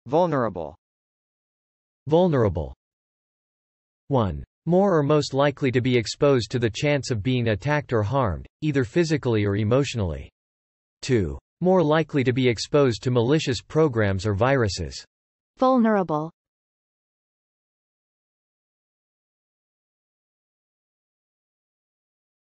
VULNERABLE - Meaning and Pronunciation
How to pronounce vulnerable? This video provides examples of an American pronunciation of vulnerable from two male and two female speakers.
In addition, it explains the meaning of vulnerable through a ditionary definition and several visual examples.

IPA Transcription of vulnerable: /vˈʌlnɚəbəl/
Definition of vulnerable: 1. More or most likely to be exposed to the chance of being attacked or harmed, either physically or emotionally.
2. More likely to be exposed to malicious programs or viruses.

If you'd like to test your pronunciation of vulnerable, check out Accent Hero's real-time pronunciation feedback tool: